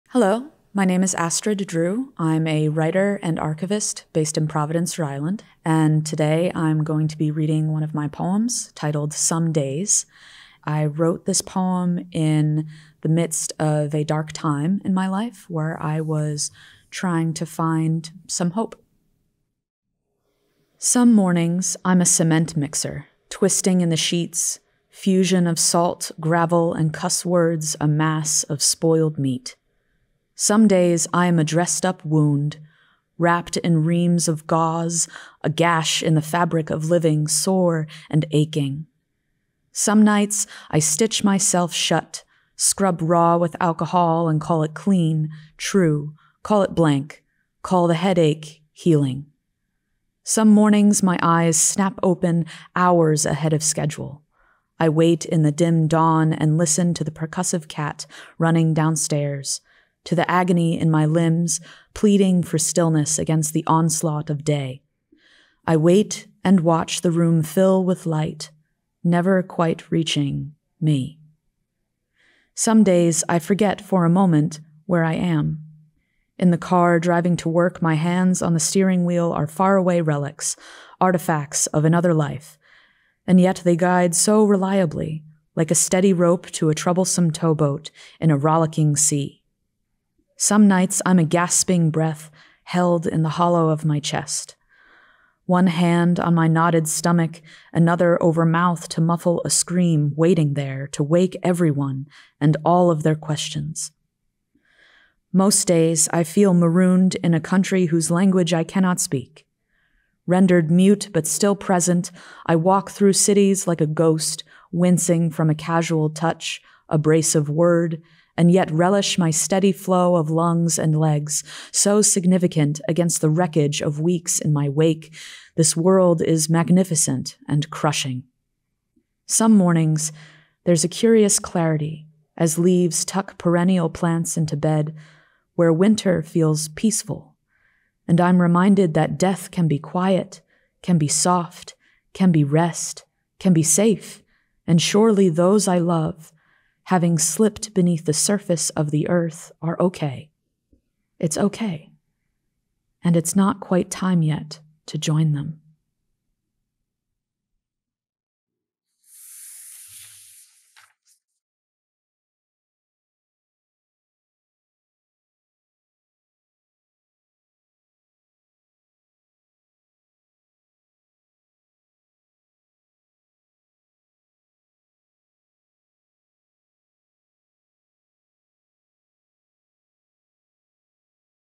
Astrid Drew reads "Some Days"
Astrid Drew (she/her/hers) reads her poem, “Some Days.”